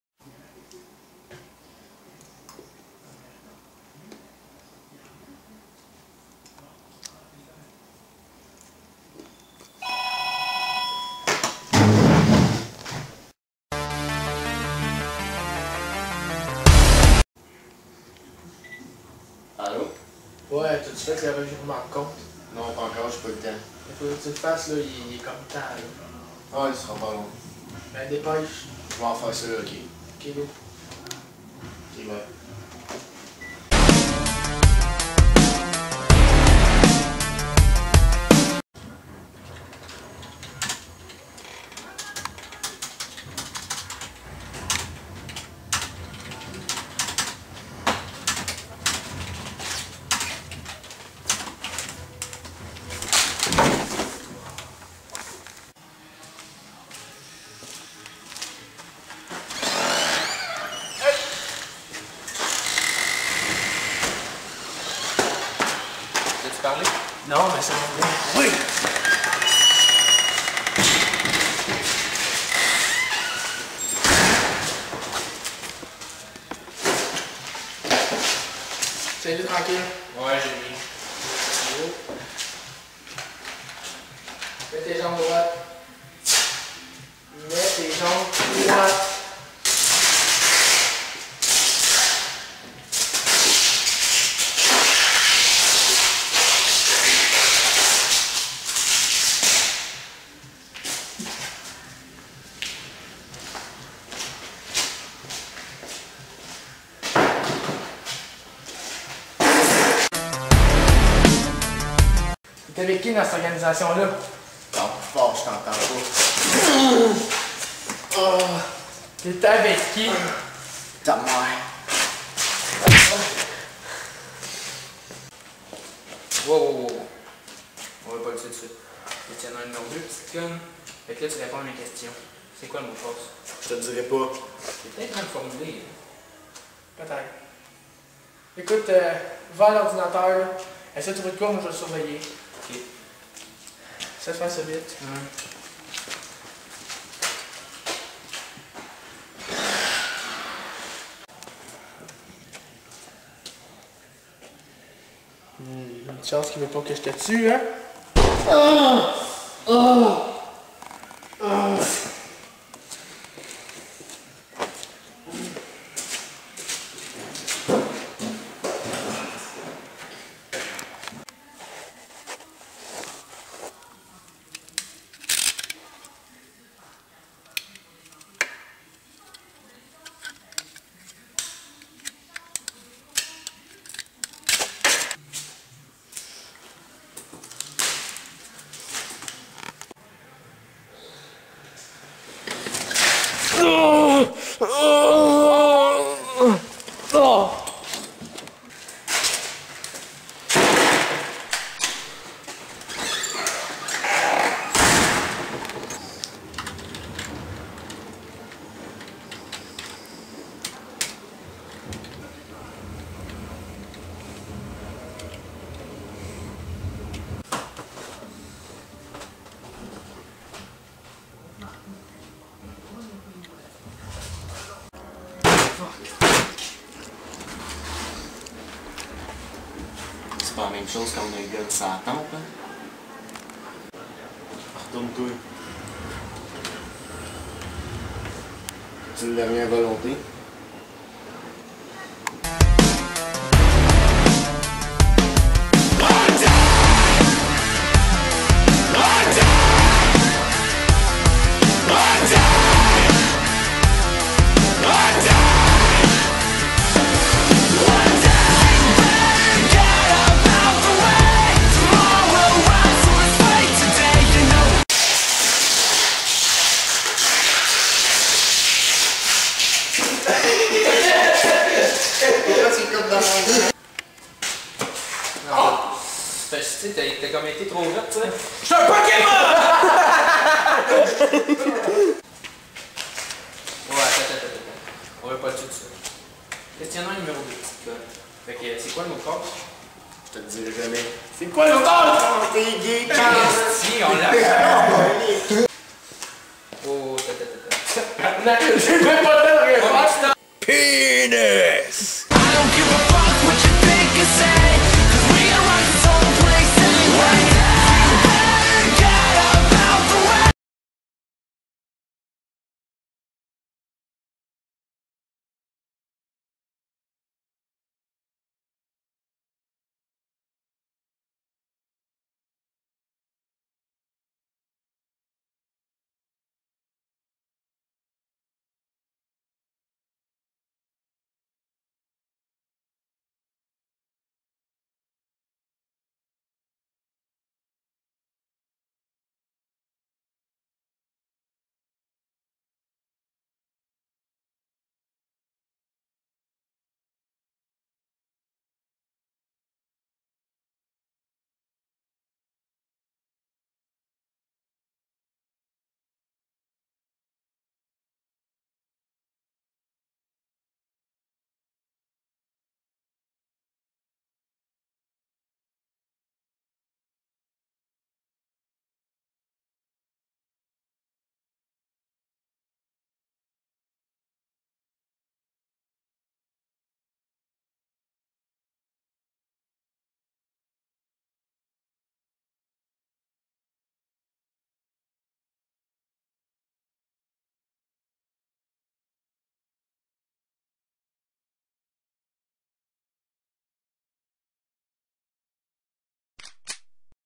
La Dernière Transaction
une transaction d'un compte de banque plein au as qui se change en intérogatiore ^^ partie 2 bientot =P alors abonner vous pour rester au courant de nos derniere videoet pour gagner au loto 6 49 ok sa cest pas sure xD
anjc prod ^^